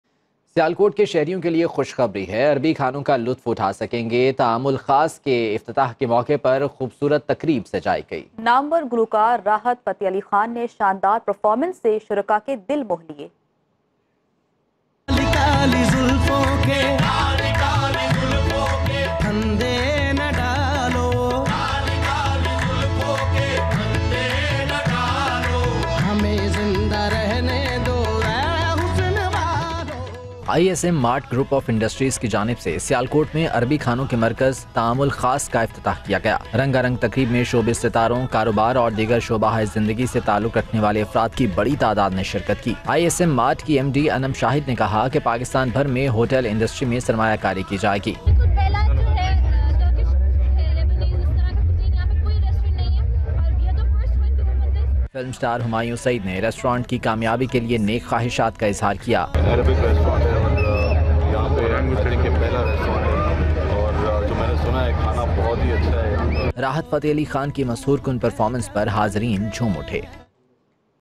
The Grand Opening of Ta'am Ul Khaas | Arabic Cuisine in Sialkot | Event organize by Ismmart
The Grand Opening of Ta'am Ul Khaas sets the stage for culinary excellence and innovation.
Renowned actor Humayun Saeed and beloved actress Aisha Omar graced the event with their presence, offering their heartfelt well-wishes to this extraordinary initiative.
The night was made even more magical with the unforgettable musical performance by the maestro, Rahat Fateh Ali Khan. His soul-stirring melodies elevated everyone passion to the roof.
Anum Shahid, the visionary Managing Director of ISMMART, spoke of boundless opportunities in the food industry, emphasizing that this is just the beginning of a delectable journey.